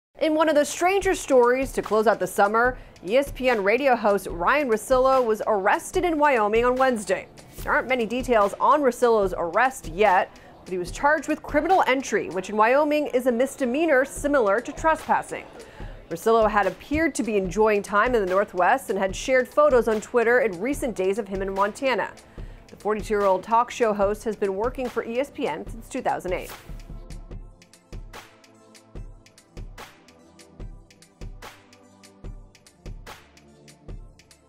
ESPN Radio Host Ryen Russillo Arrested In Wyoming For Criminal Entry | SI Wire | Sports Illustrated
Ryen Russillo, the host of The Russillo Show on ESPN Radio, was arrested in Jackson, Wy., on Wednesday for misdemeanor criminal entry, a charge that is similar in nature to trespassing.